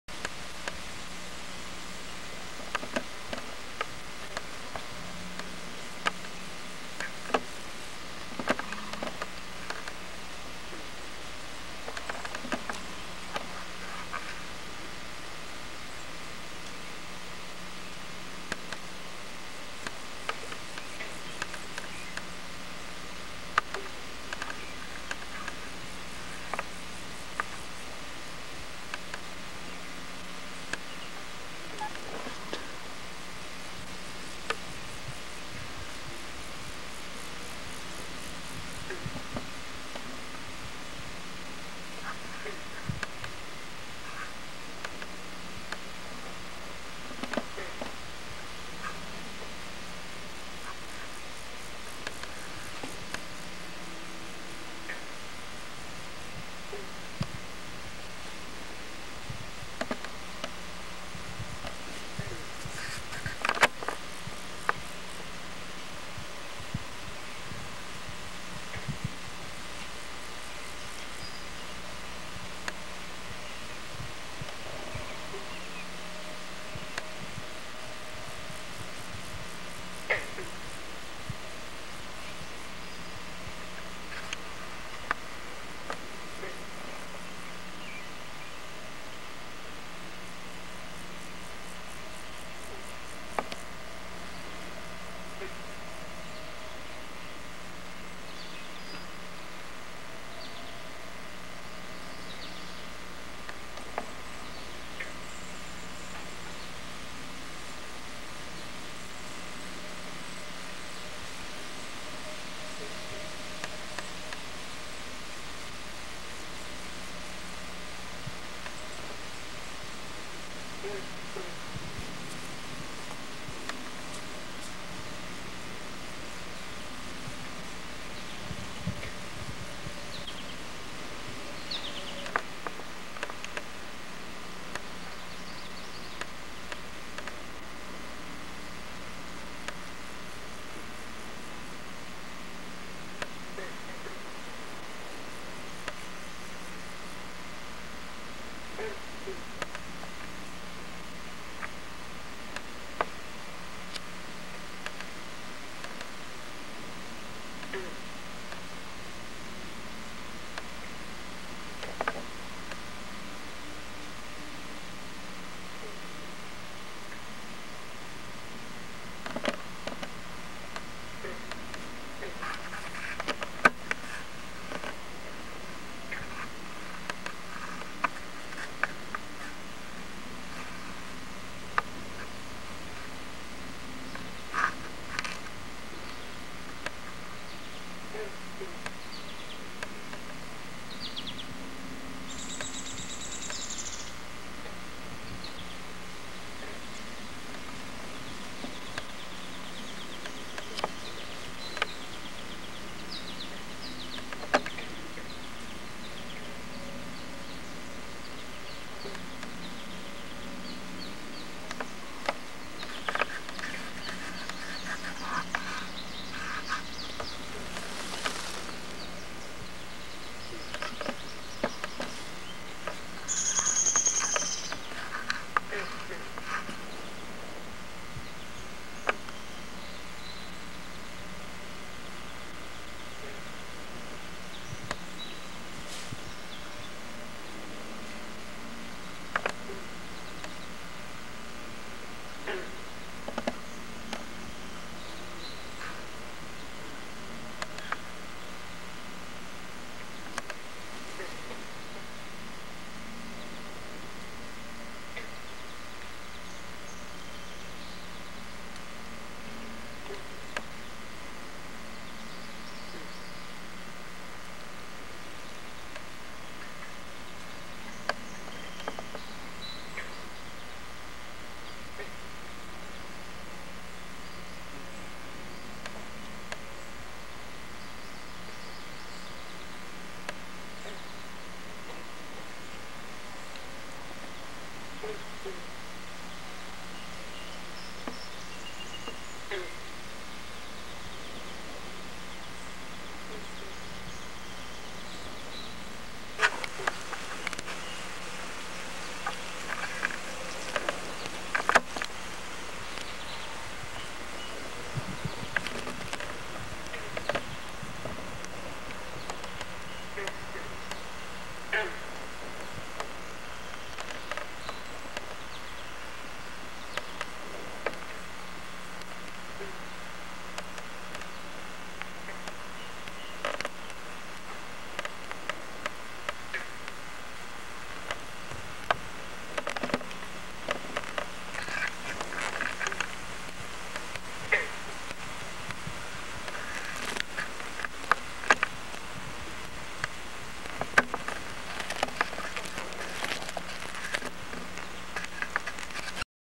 otter mom and pups roll on back
in July when the pups are a little over three months old